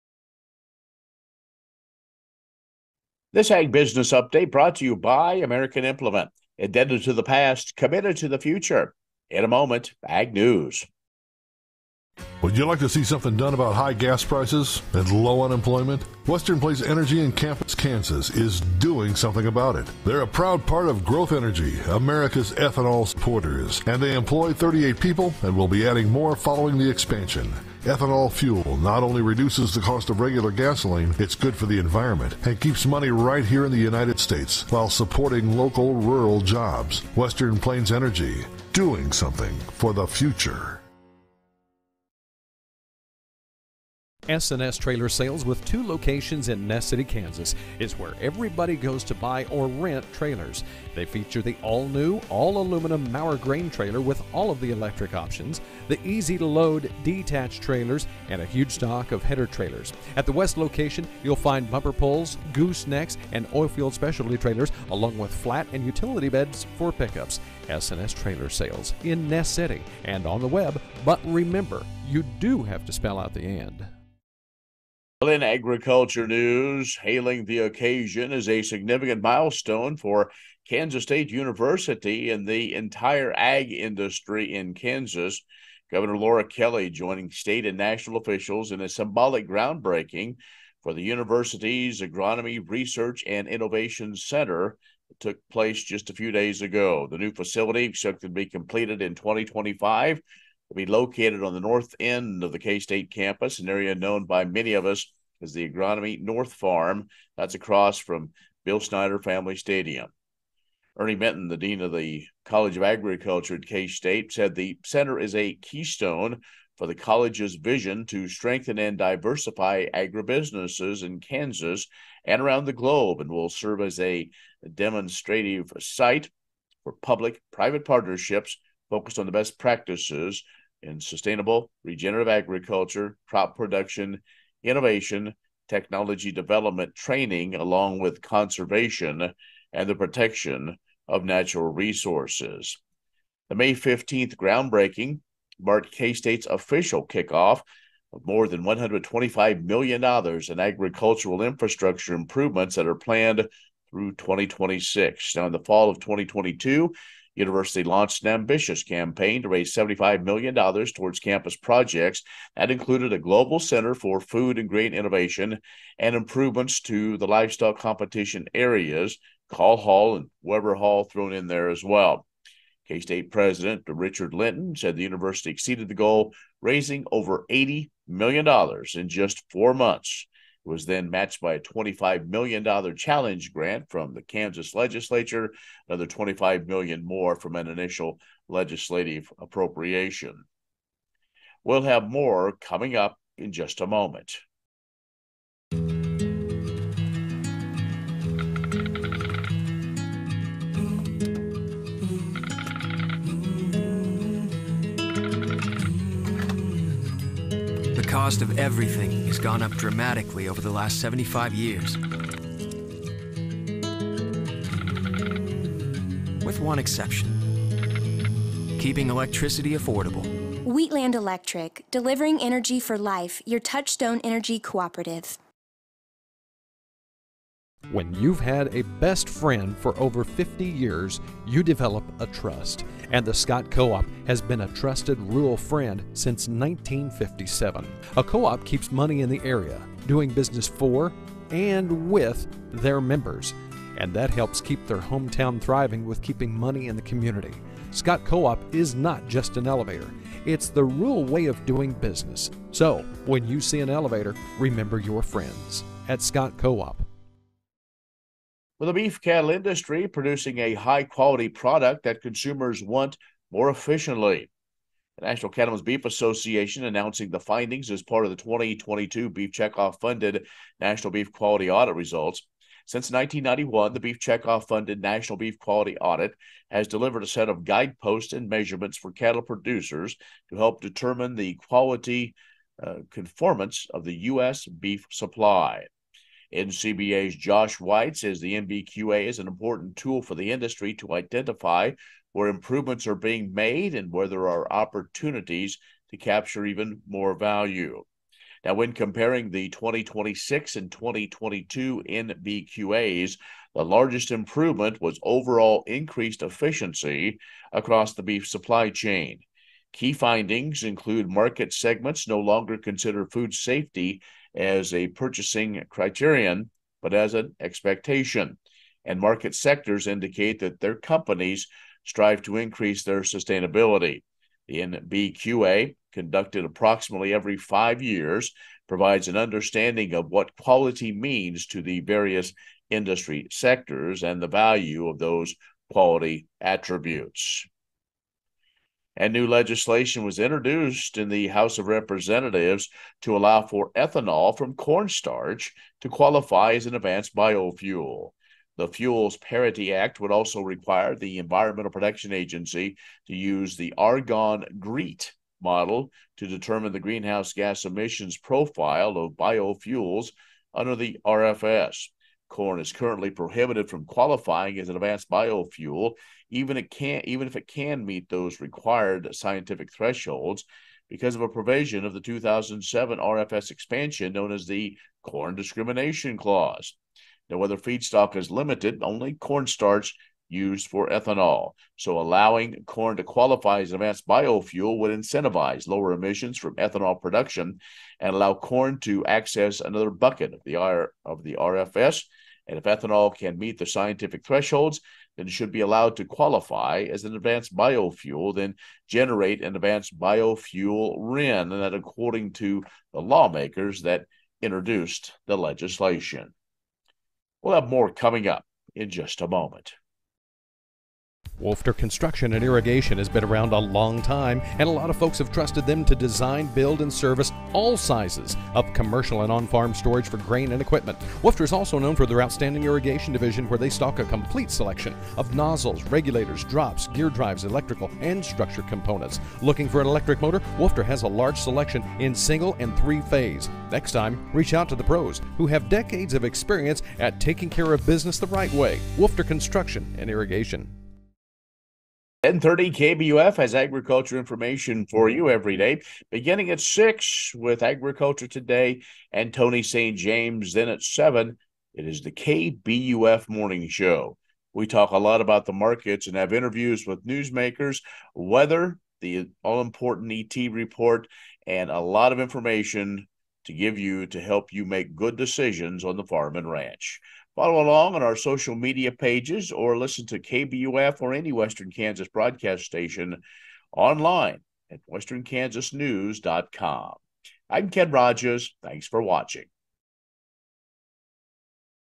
KBUF Ag News - May 19, 2023
Ken Rahjes with an ag news update